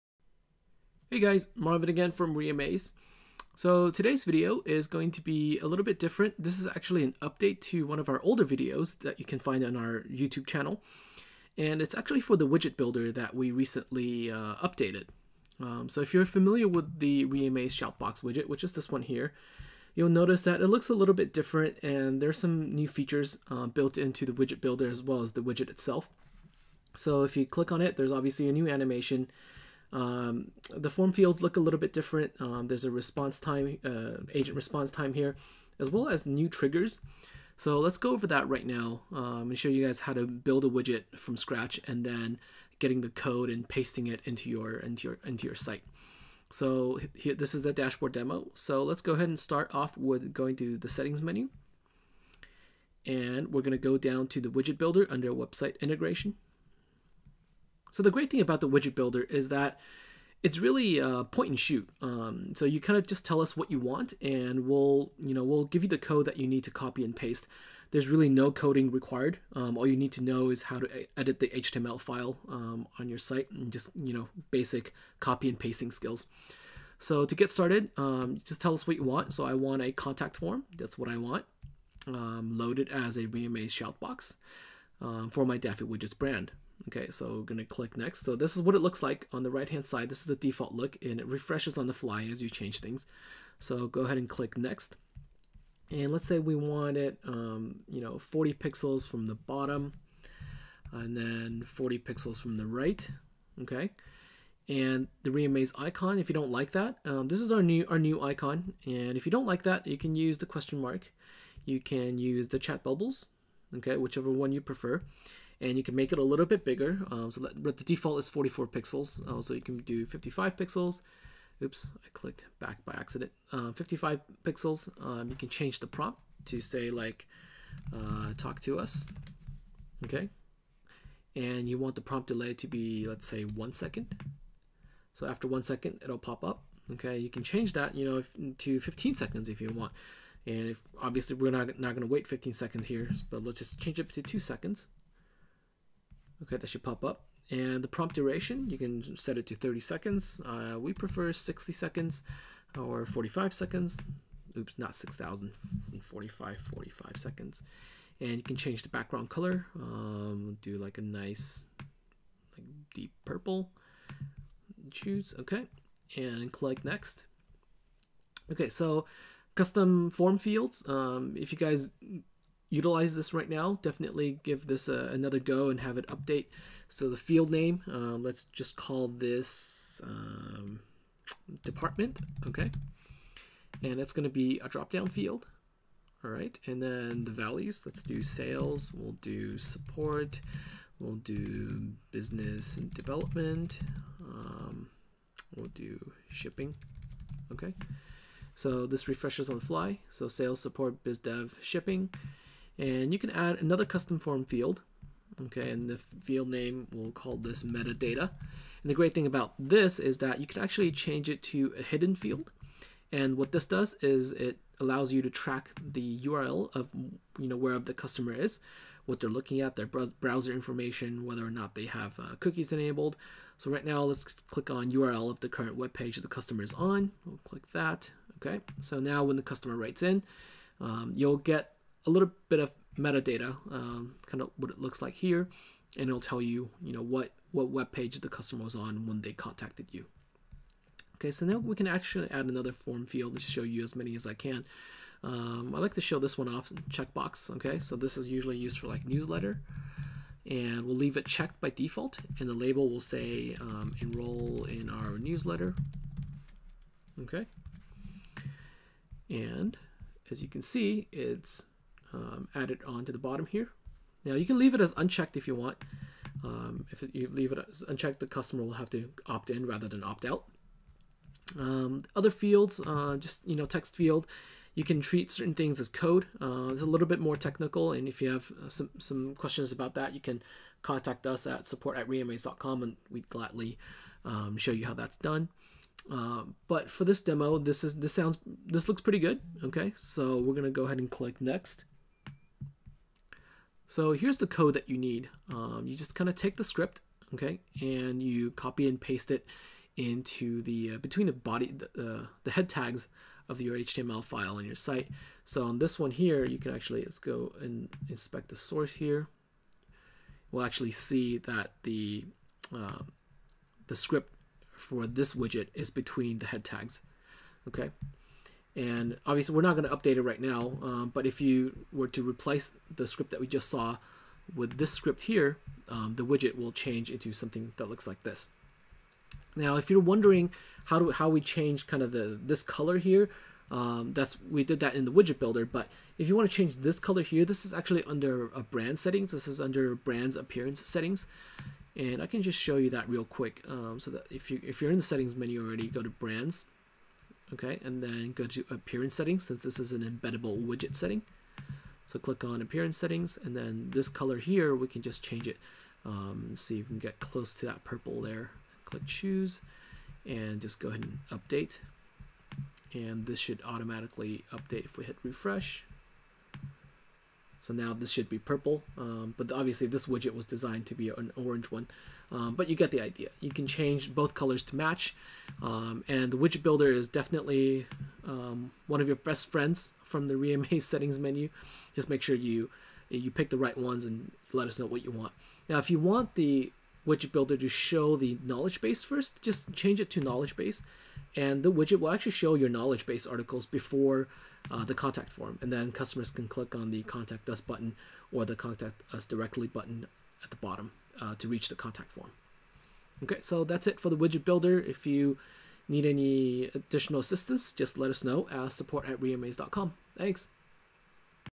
Reamaze Tutorial - Widget Builder Update
The Reamaze widget builder has been updated with new features such as timed prompts, new icon triggers, and agent response times. Check it out!